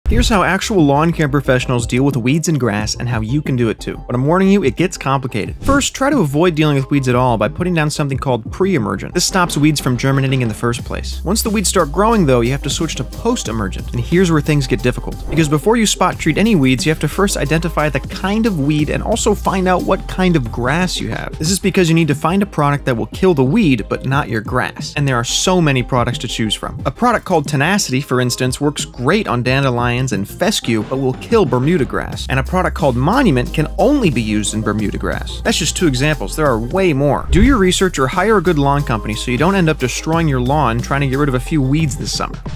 How to Get Rid of Weeds in Your Lawn
How to get rid of weeds in your grass like a professional lawncare company. There are so many post emergent weed control products on the market but the main things you have to know are what kind of grass you have and what kind of weeds you have and choose a product accordingly